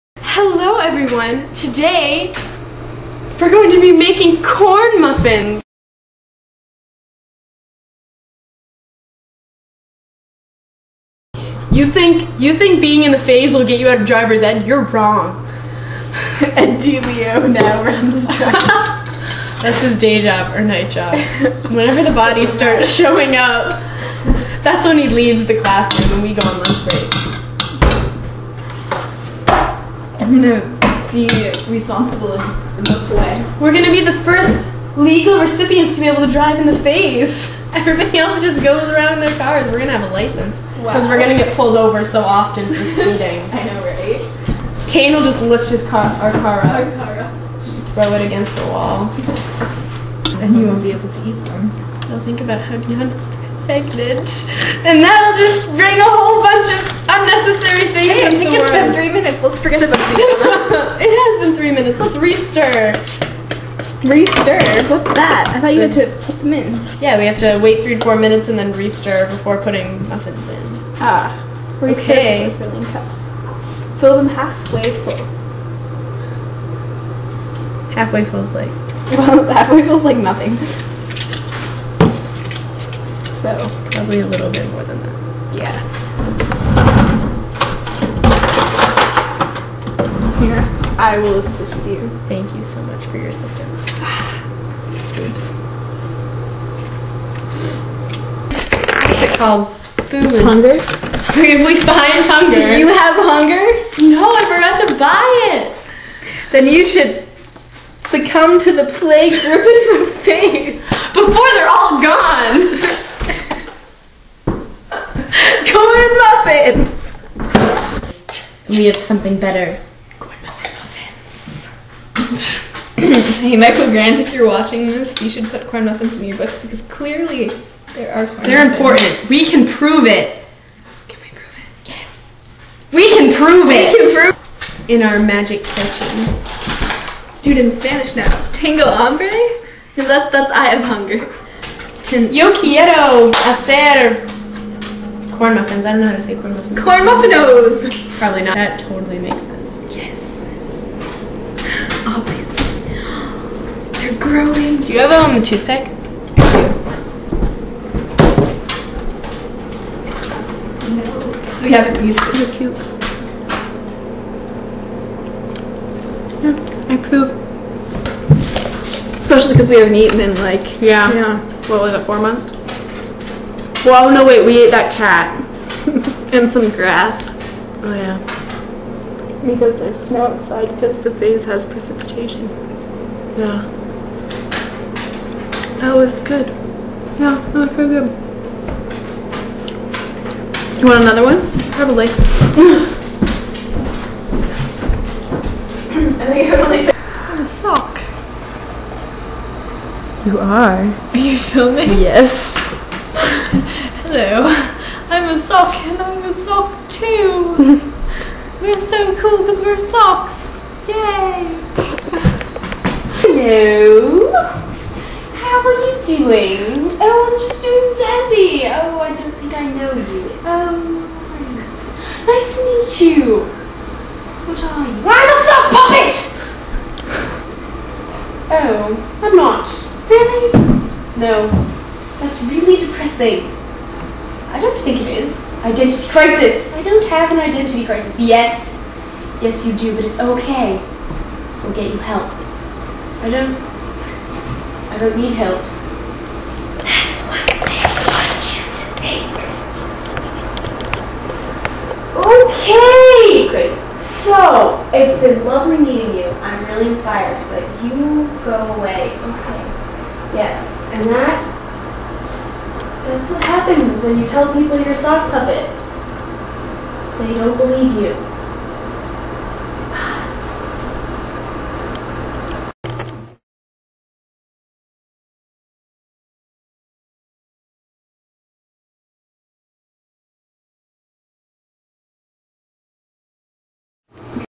Corn Muffins
Some residents of the FAYZ (We, the CoatesAcademy-ians) make some corn muffins to save us from starving to death. And there's some other random stuff.
There's been a lot of people commenting on videos lately and you're all really awesome! So thanks and sorry we haven't been uploading much.

O btw if you don't get the corn muffins thing, here's a facebook group on the subject that should clear up any confusion
Thanks everyone for watching us~ :D